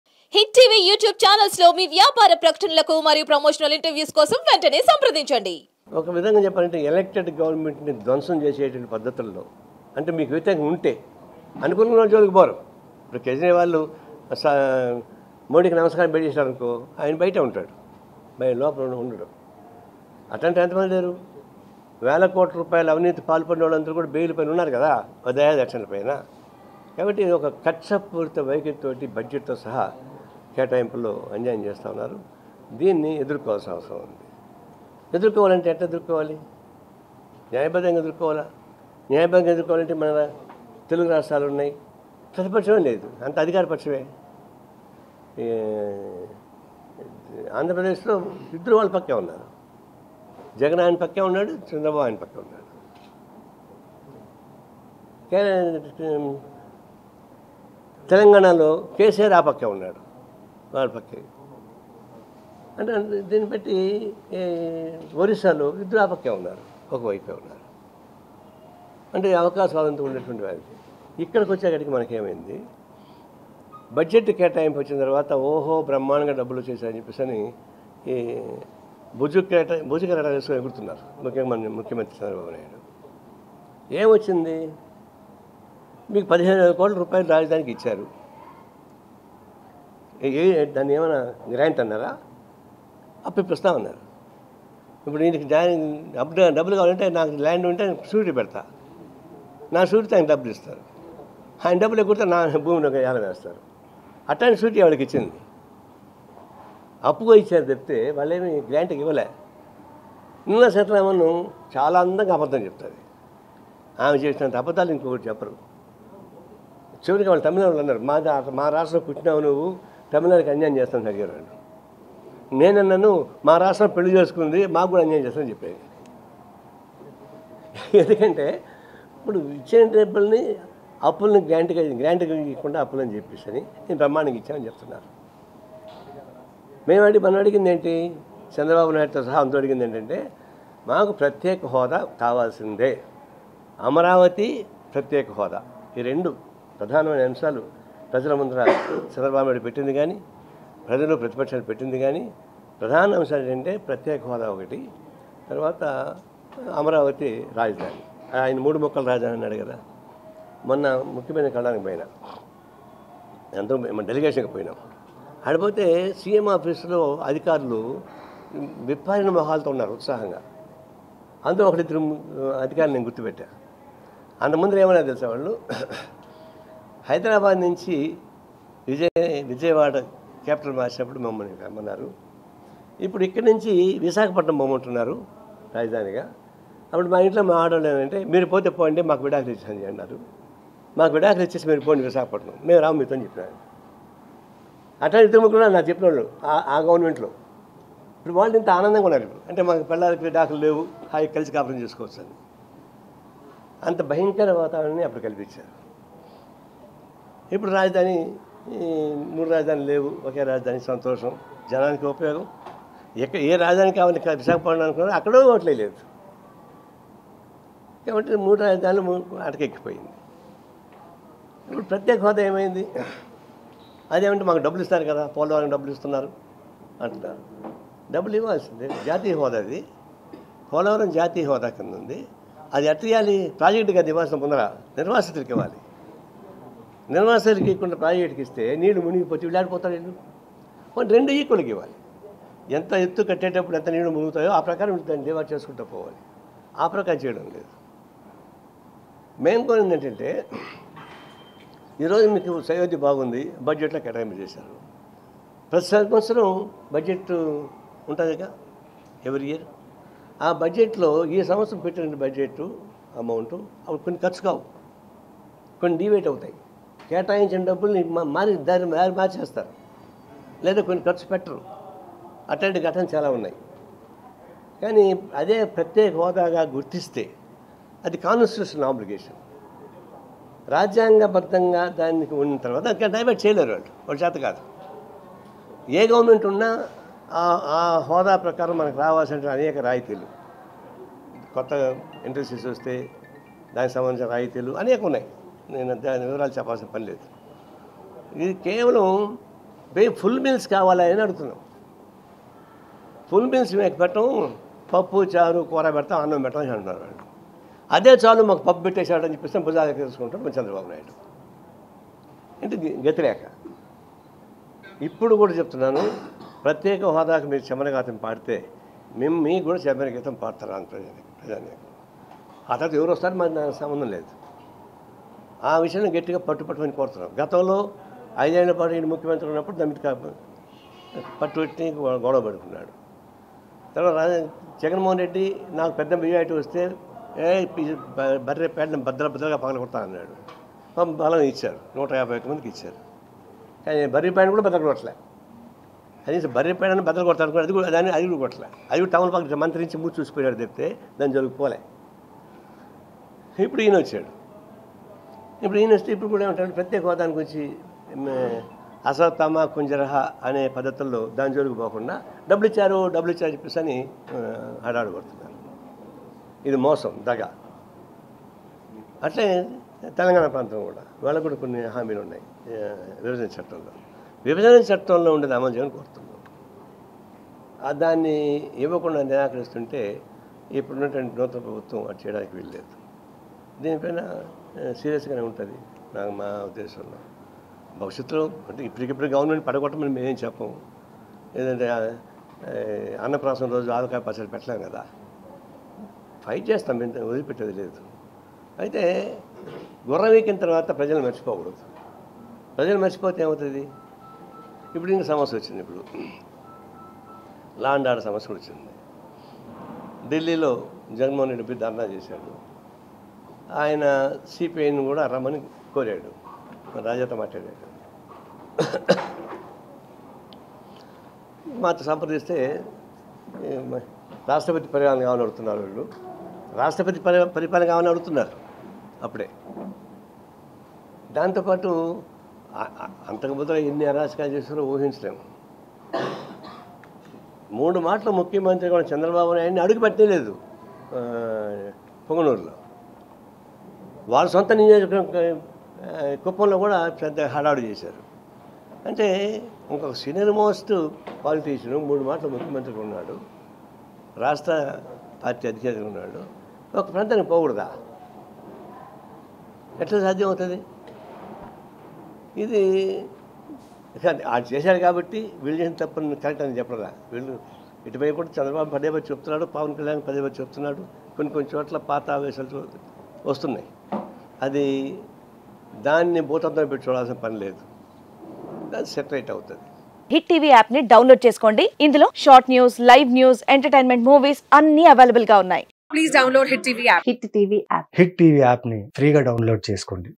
మూడు రాజధానులు అటకెక్కిపోయినయ్.. | CPI Narayana Shocking Comments On Jagan and Chandra Babu
Watch : మూడు రాజధానులు అటకెక్కిపోయినయ్.. | CPI Narayana Shocking Comments On Jagan and Chandra Babu  @Hittvtelugunews